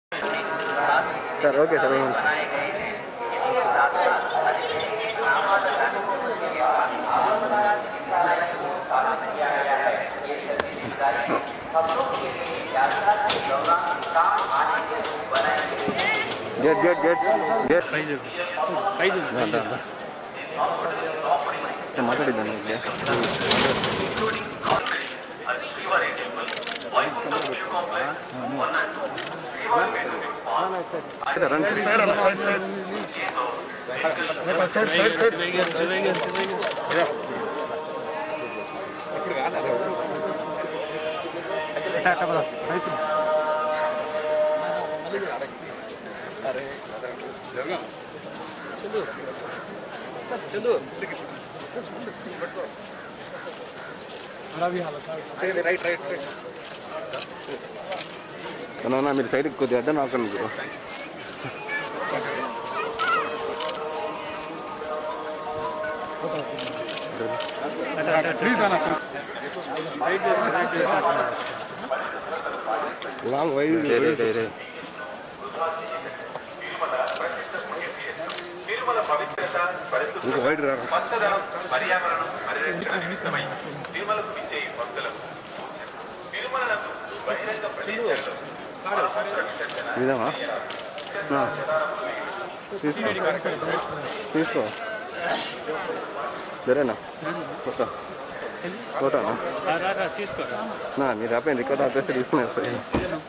Hero Nithin Visits Tirumala Temple With Check Movie Team | Check Movie Success | Andhra Life Tv
Watch Here :Hero Nithin Visits Tirumala Temple With Check Movie Team